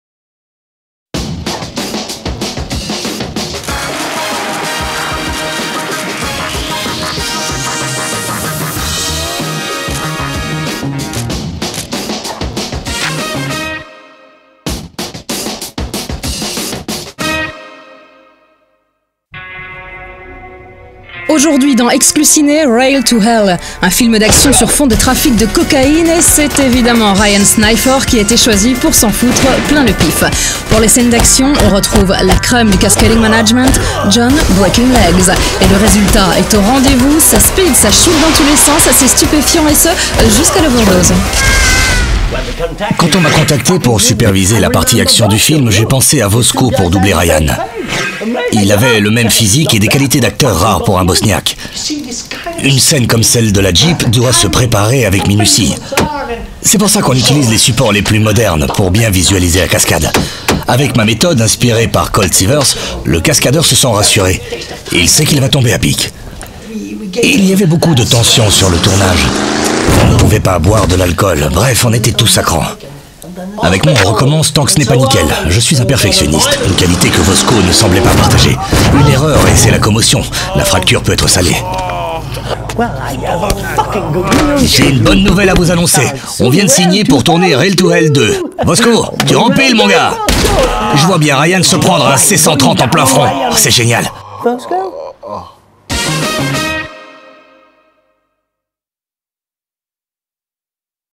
Exclus Ciné : La doublure
« Exclus Ciné » est une web série humoristique de type court, environ deux minutes par épisode. Elle parodie des émissions traitant des sorties cinéma et leur promotion excessivement positive.
Au passage, elle égratigne également les lieux communs des différents genres cinématographiques ainsi que les obsessions de tous ces passionnés œuvrant dans les nombreux métiers du cinéma.